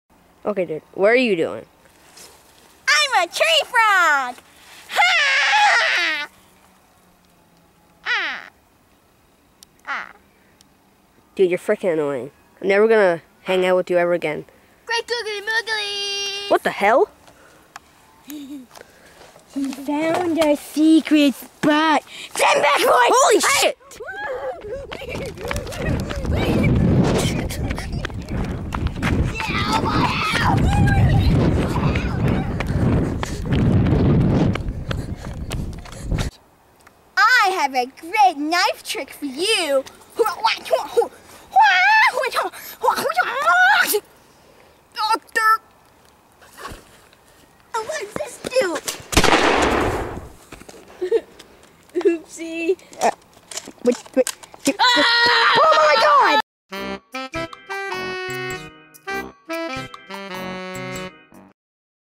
Treefrog (Juice Boxers Video)
Tanner thinks he is a treefog, and Liam thinks he is a hobbo.